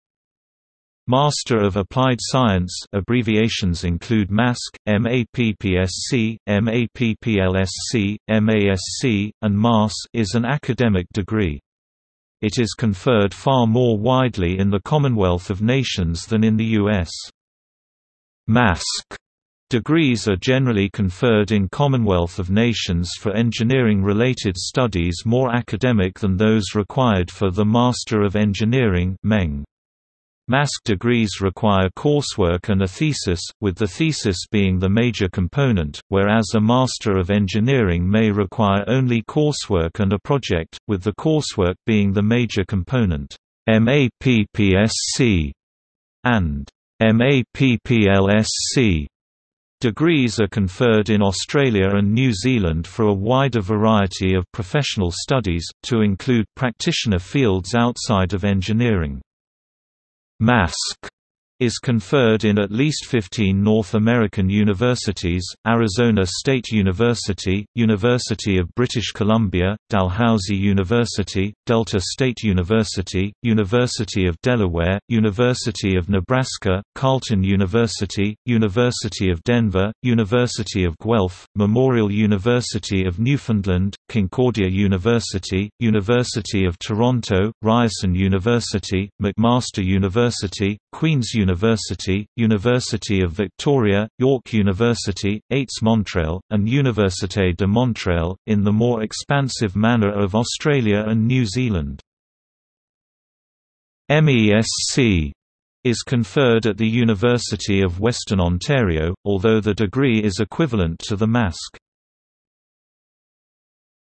Master of Applied Science | Wikipedia audio article
- Socrates

SUMMARY
Master of Applied Science (abbreviations include MASc, MAppSc, MApplSc, M.A.Sc. and MAS) is an academic degree. It is conferred far more widely in the Commonwealth of Nations than in the US.
"MASc" degrees are generally conferred in Commonwealth of Nations for engineering-related studies more academic than those required for the Master of Engineering (MEng). MASc degrees require coursework and a thesis, with the thesis being the major component, whereas a Master of Engineering may require only coursework and a project, with the coursework being the major component."MAppSc" and "MApplSc" degrees are conferred in Australia and New Zealand for a wider variety of professional studies, to include practitioner fields outside of engineering.
"MESc" is conferred at The University of Western Ontario, although the degree is equivalent to the MASc.